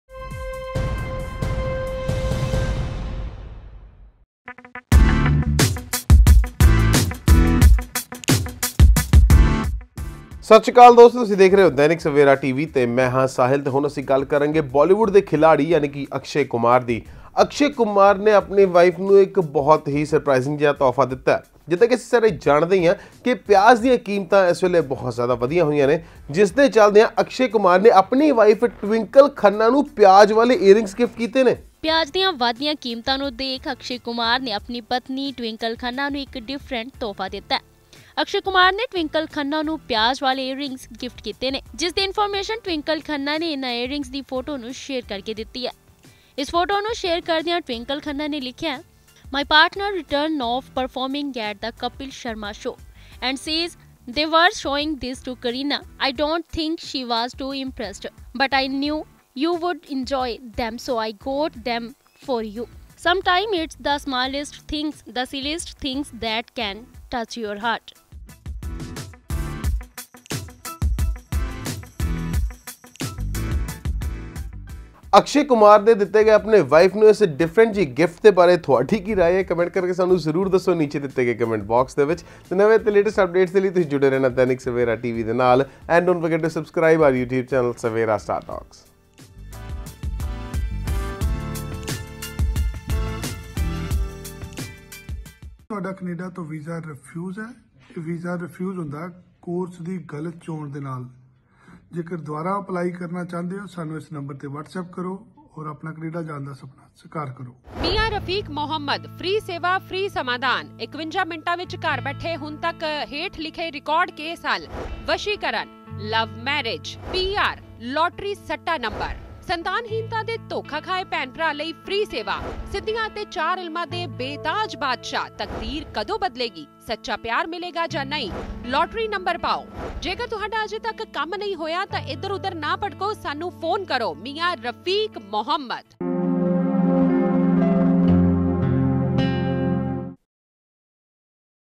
ਪਿਆਜ ਦੇ ਵਧਦੇ ਭਾਅ ਦੇਖ Akshay Kumar ਨੇ ਦਿੱਤਾ ਆਪਣੀ ਪਤਨੀ ਨੂੰ ਇਹ Gift |Dainik Savera
ਪਿਆਜ ਦੇ ਵਧਦੇ ਭਾਅ ਦੇਖ Akshay Kumar ਨੇ ਦਿੱਤਾ ਆਪਣੀ ਪਤਨੀ ਨੂੰ ਇਹ Gift
|Dainik Savera